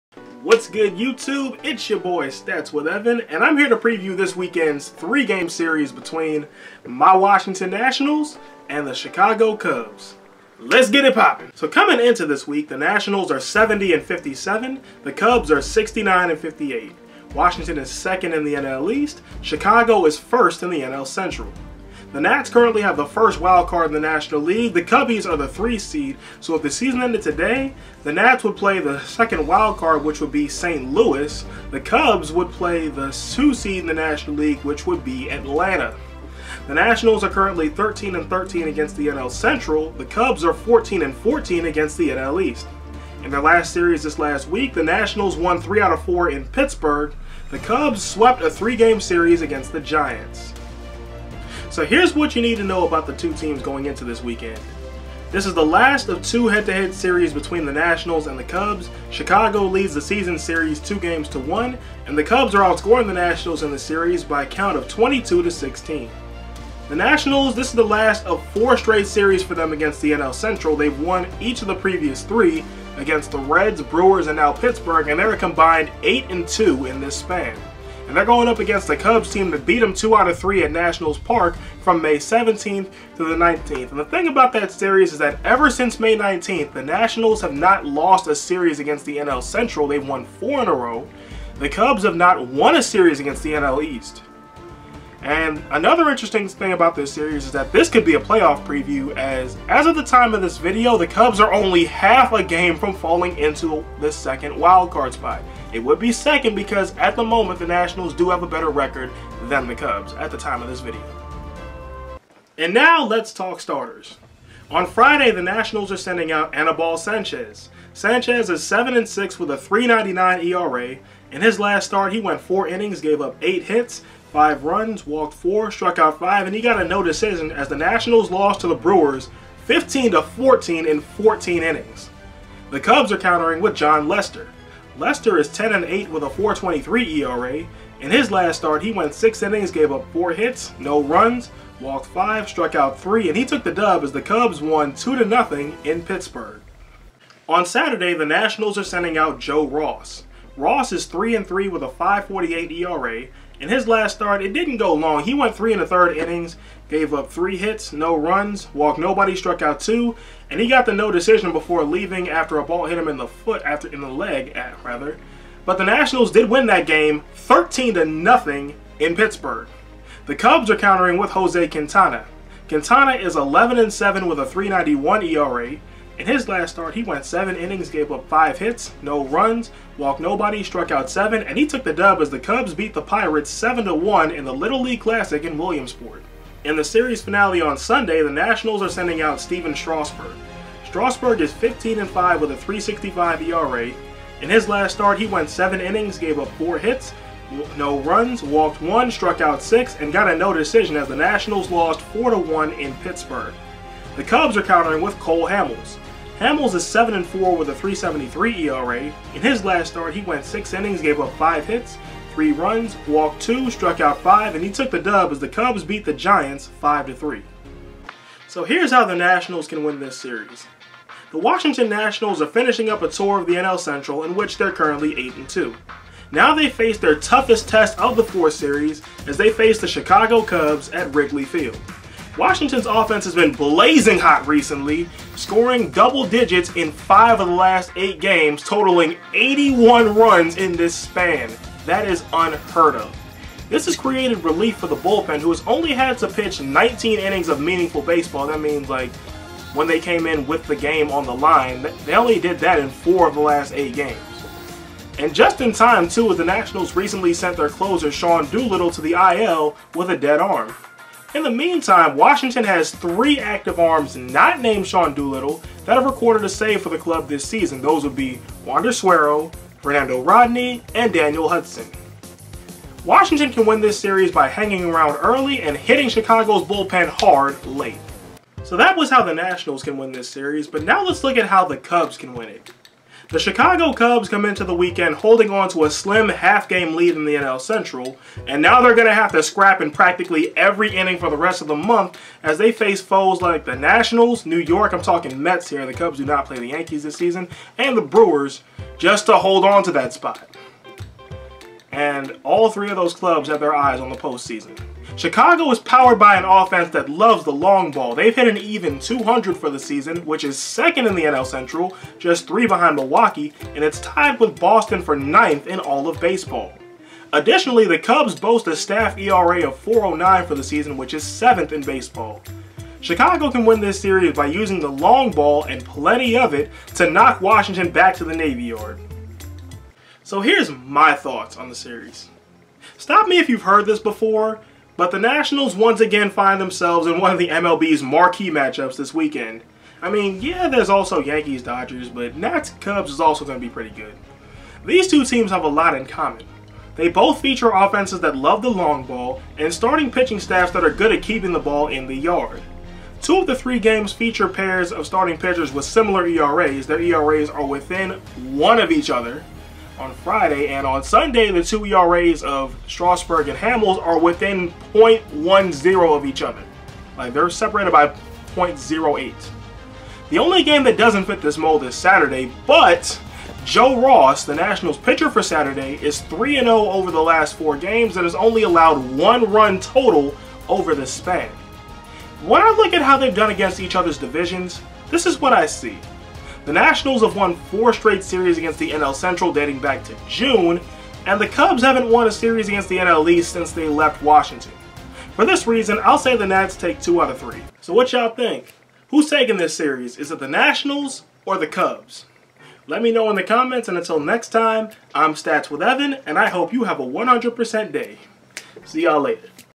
Nationals @ Cubs Preview 8/23-25
I take a look at the Washington Nationals series this weekend as they head to Chicago to take on the Cubs.

Instagram and Twitter: @thevoiceofevan